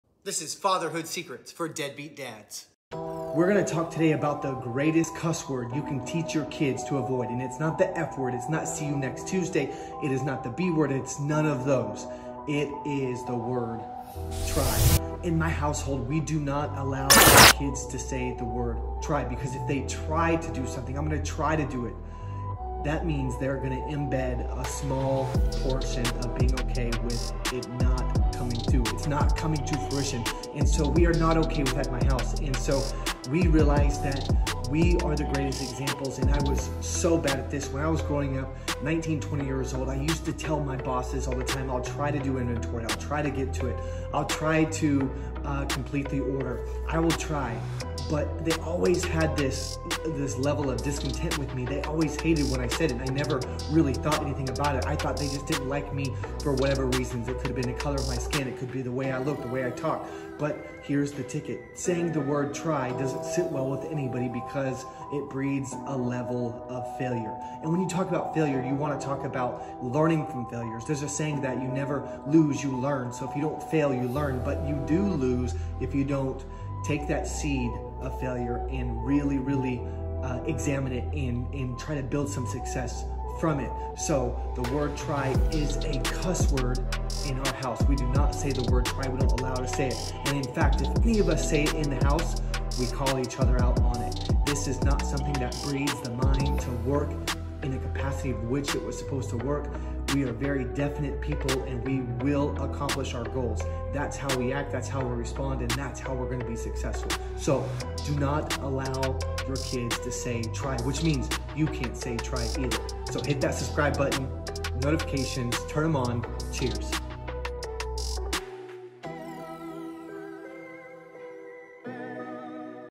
The One Word You Should Never Say in Front of Your Children
In this thought-provoking video, we discuss the impact and potential consequences of using profanity in front of your kids. We uncover the power of words and delve into the significance of one particular word that should never be uttered in your children's presence. Children are highly impressionable, and as parents, it's crucial to be mindful of the language we use around them. Our aim is to shed light on the detrimental effects of exposing young minds to inappropriate language and to encourage healthier ways of communication within the family. Through insightful discussions and expert interviews, we explore the potential long-term impacts of using the worst cuss word in front of your children. We analyze its potential influence on their language development, psychological well-being, and overall behavior. By considering alternative approaches to expressing frustration or anger, we hope to empower parents to foster an environment of positivity, respect, and mindfulness. Together, we can shape a future where children are shielded from harmful language and equipped with the tools to communicate effectively.